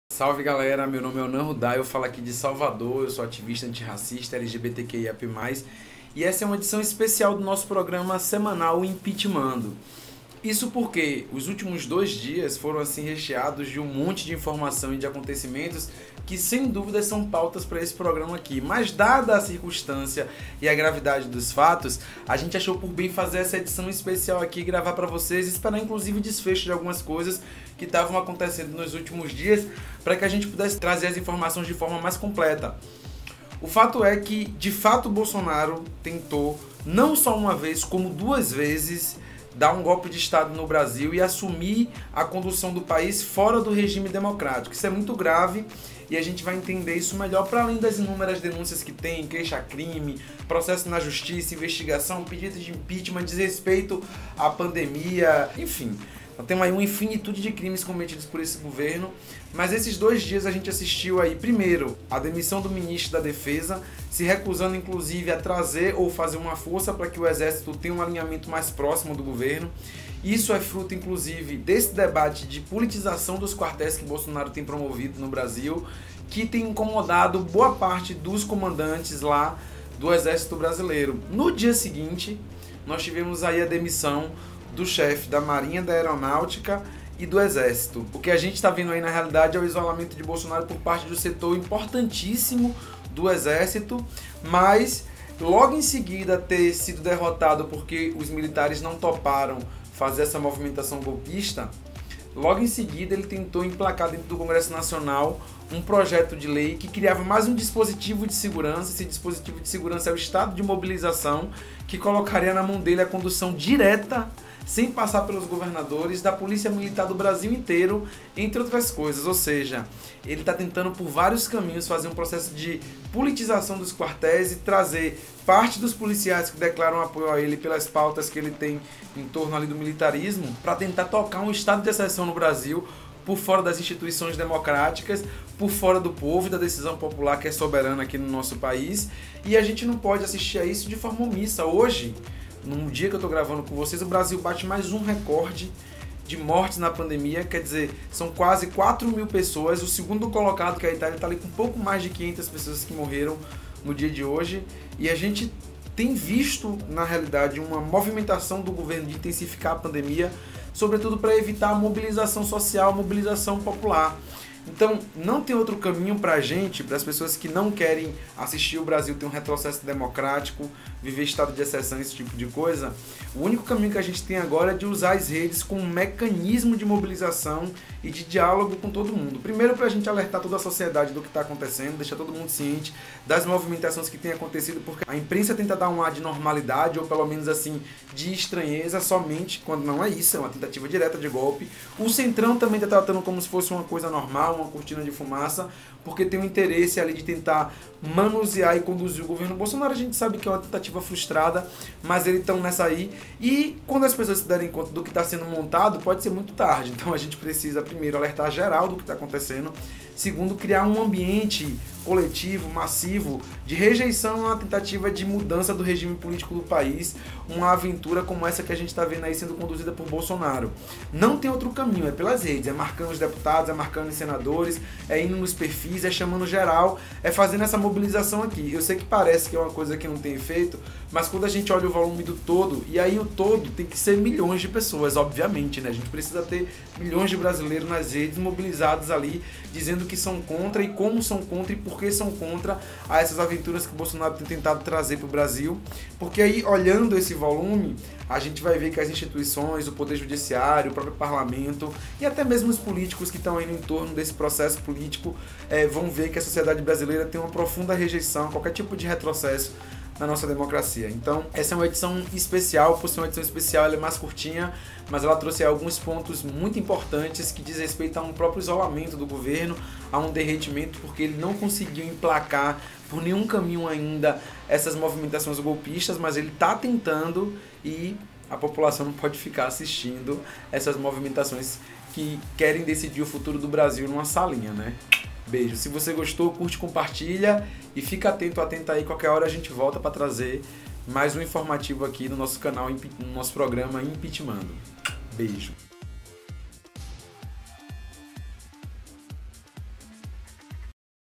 Impeachmando - Bolsonaro está tentando dar um golpe? Entenda
Nessa edição especial do Impeachmando, Onã repassa as recentes instabilidades de Bolsonaro com o Ministério da Defesa e as Forças Armadas. Confira